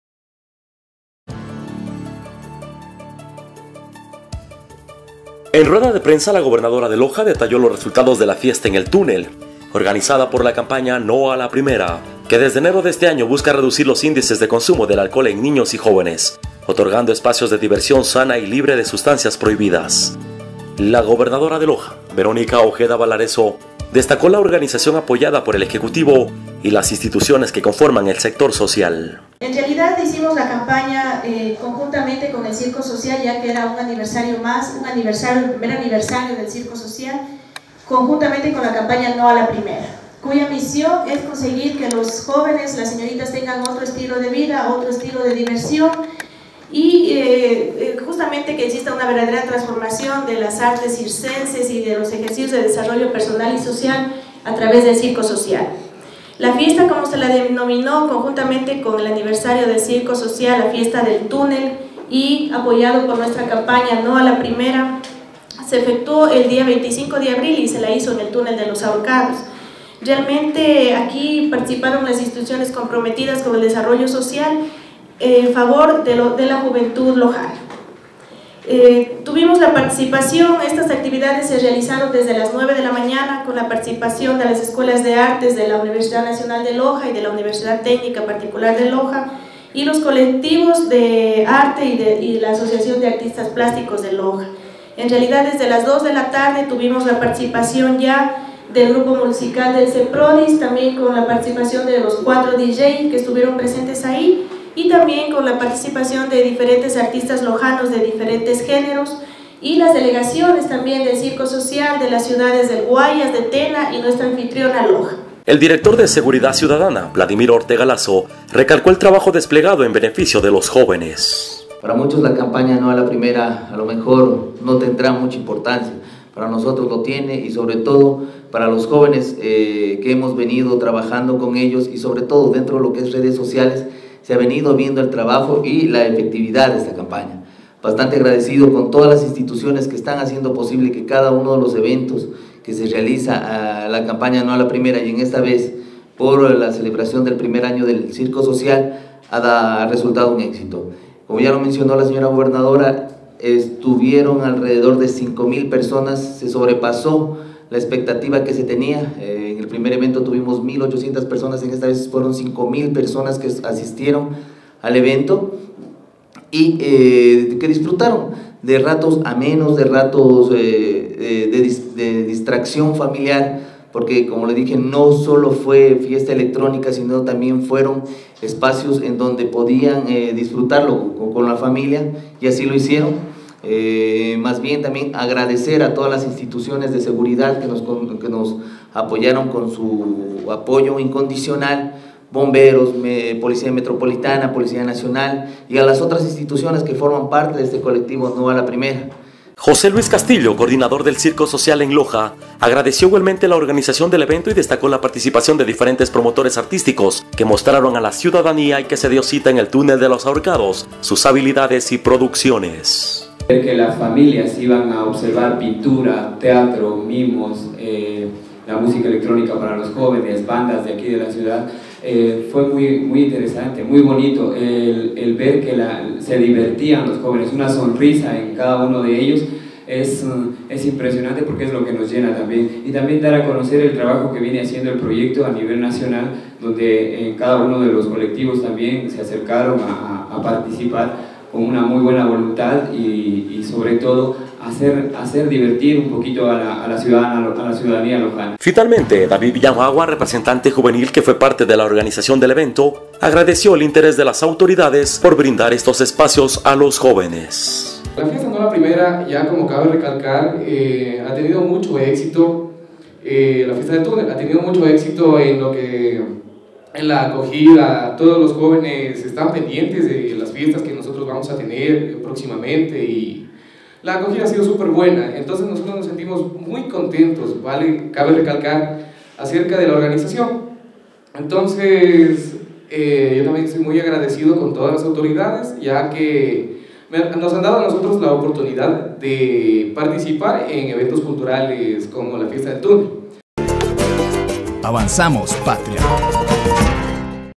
Resultados de la fiesta en el túnel, organizada por la campaña No a la Primera
En rueda de prensa, la Gobernadora de Loja detalló los resultados de la Fiesta en el Túnel, organizada por la campaña No a la Primera, que desde enero de este año busca reducir los índices de consumo de alcohol en niños y jóvenes, otorgando espacios de diversión sana y libre de sustancias prohibidas.

La Gobernadora de Loja, Verónica Ojeda Valarezo, destacó la organización apoyada por el Ejecutivo y las instituciones que conforman el sector social, con la finalidad de celebrar el primer año del Circo Social en Loja, permitiendo a la ciudadanía en general disfrutar de espectáculos circenses, artísticos y musicales, en un evento que duró desde las 9 de la mañana hasta las 10 de la noche del viernes 25 de abril.

El Director de Seguridad Ciudadana de la Gobernación, Wladimir Ortega Lasso, manifestó: "Para nosotros y para los jóvenes con los que hemos trabajado esta campaña es muy importante por la efectividad que ha alcanzado y los resultados que estamos teniendo, ofreciendo a toda la familia espacios para disfrutar sanamente."

José Luis Castillo, Coordinador del Circo Social en Loja agradeció igualmente la organización del evento y destacó la participación de diferentes promotores artísticos que mostraron a la ciudadanía que se dio cita al túnel de los ahorcados sus habilidades y producciones.

Finamente, David Villamagua, representante juvenil que fue parte de la organización del evento, habló a nombre del colectivo de Dj que integra, agradeciendo el interés de las autoridades por brindar estos espacios a los jóvenes.

Como datos adicionales, se informó que alrededor de 5000 personas asistieron a este evento, se brindó seguridad con 1 ambulancia de la cruz roja, 6 elementos de la Policía Metropolitana del cantón Loja, 3 bomberos y 25 elementos de la Policía Nacional.